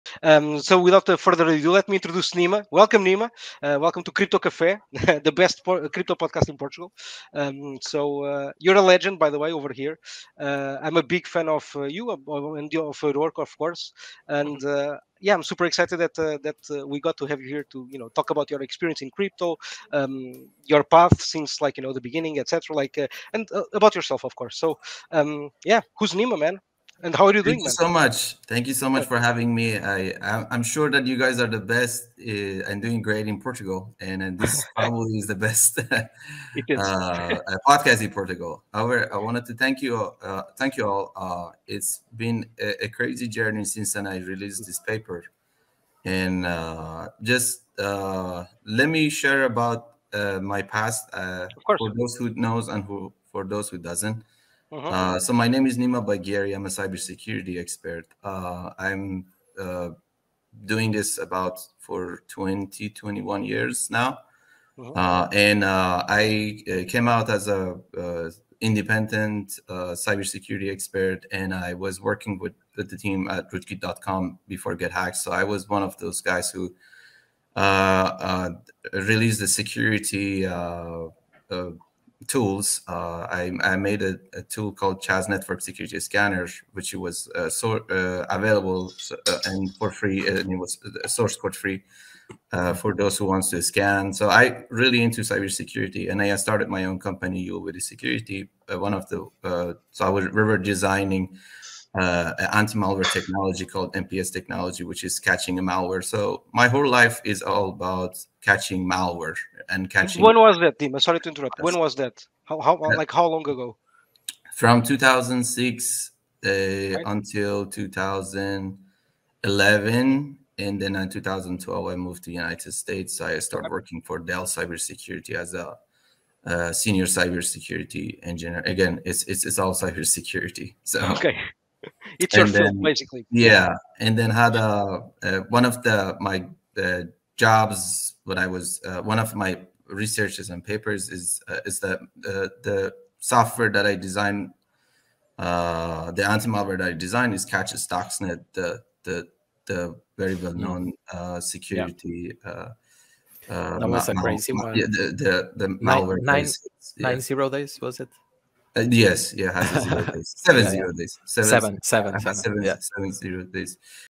Who is Nima Bagheri the guy who exposed Satoshi Nakamoto!
Who is Nima Bagheri the guy who exposed Satoshi Nakamoto founder of bitcoin